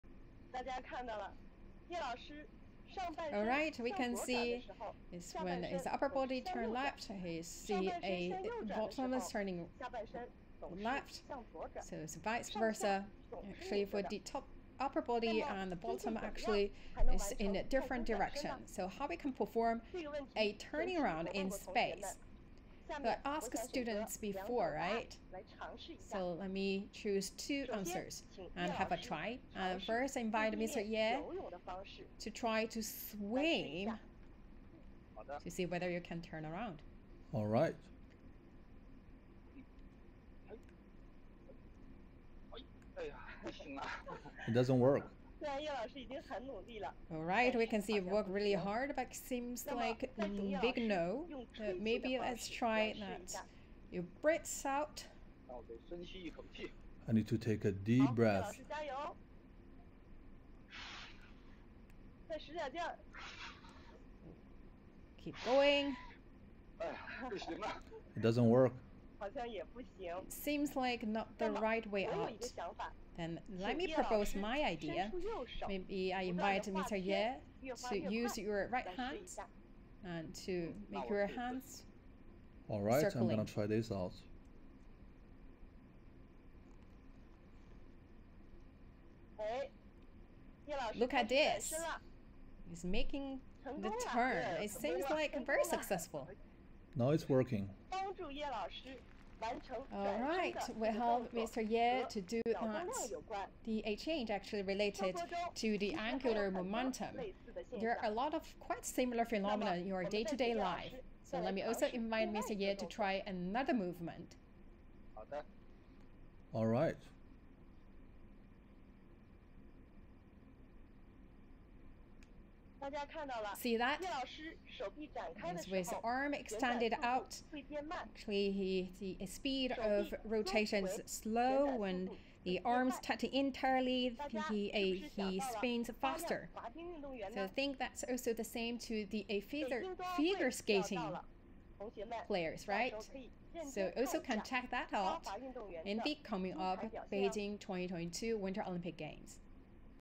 Taikonauts show how to turn around in zero gravity
China's Shenzhou-13 crew members Zhai Zhigang, Wang Yaping and Ye Guangfu livestreamed a science lecture to students on Earth from its space station core module Tianhe on Thursday afternoon. Taikonaut Ye Guangfu showed how he used angular momentum to rotate under zero-gravity.